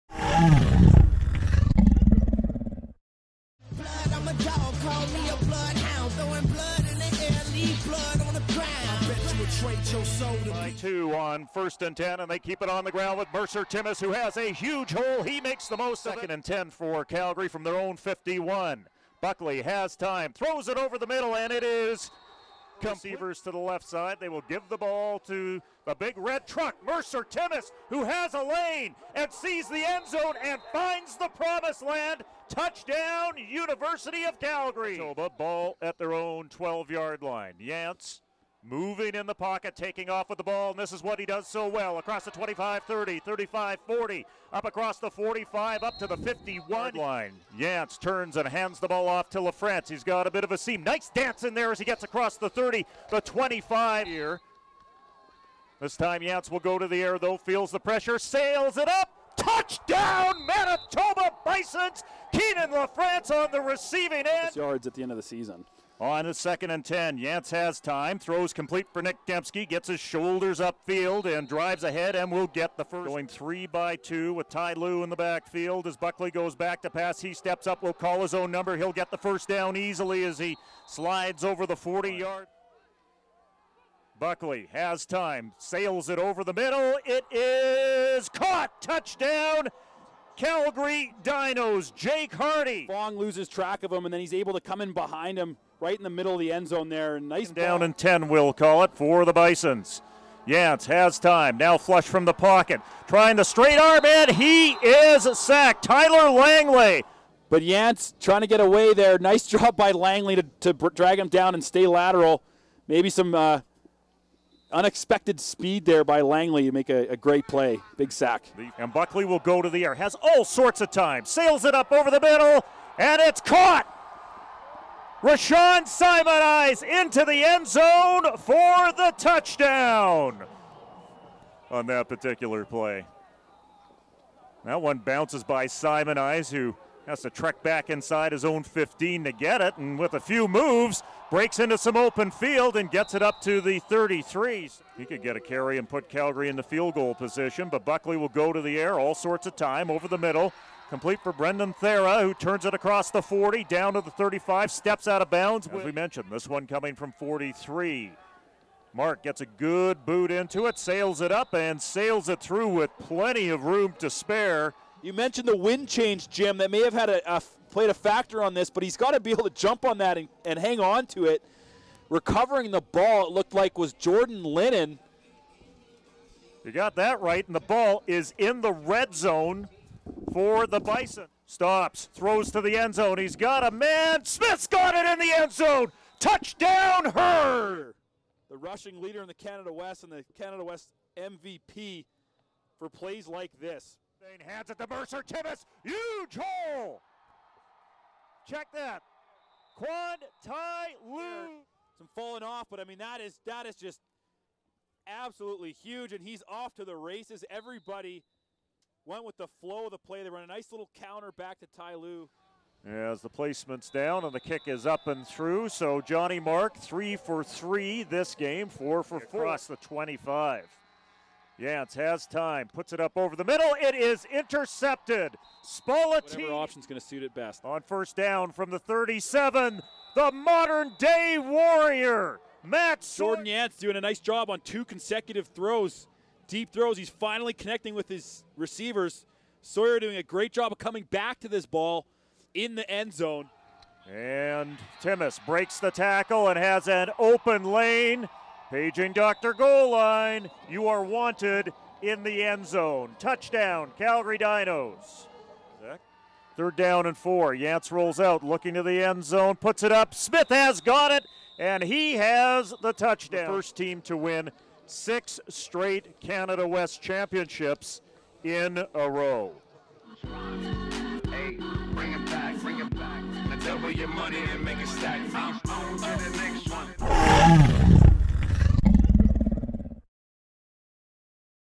FB: 77th Hardy Cup Highlights, Calgary 43 Manitoba 28
Highlights of the 77th Hardy Cup at McMahon Stadium in Calgary as the University of Calgary Dinos captured their sixth straight Canada West championship with a 43-28 win over the Manitoba Bisons Saturday, Nov. 9, 2013